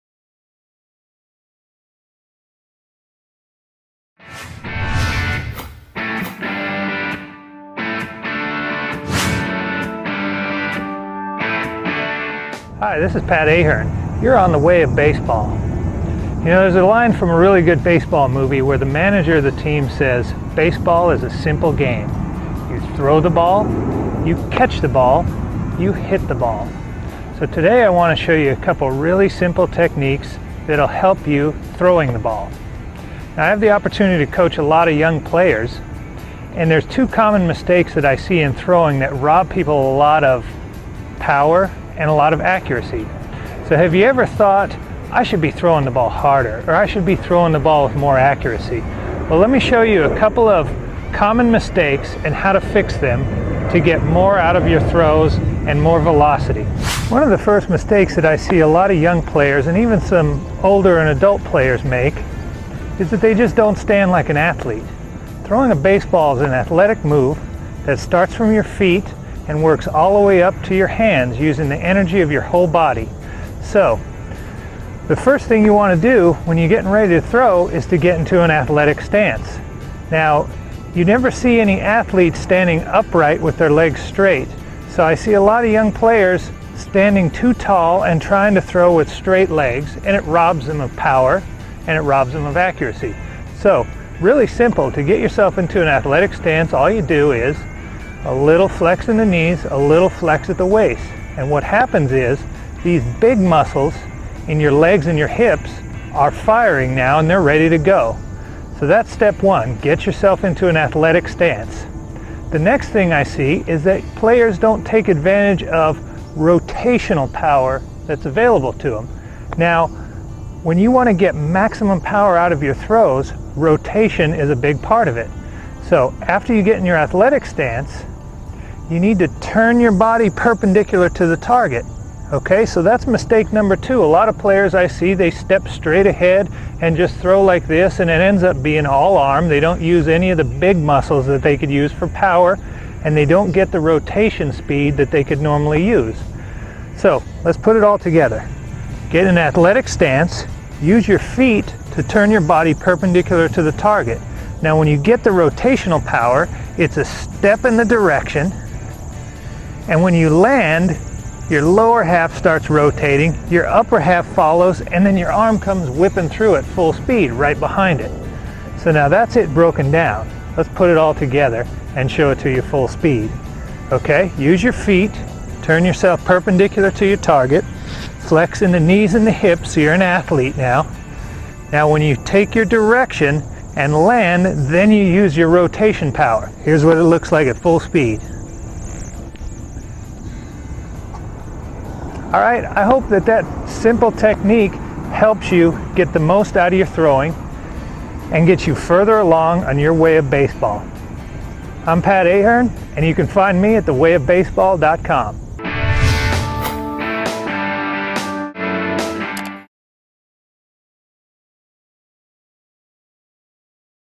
Fundamental Throwing Technique
Pat Ahearne, former NCAA College World Series Championship pitcher with more than 16 years of pro ball experience shares his fundamental throwing technique tips for youth baseball players.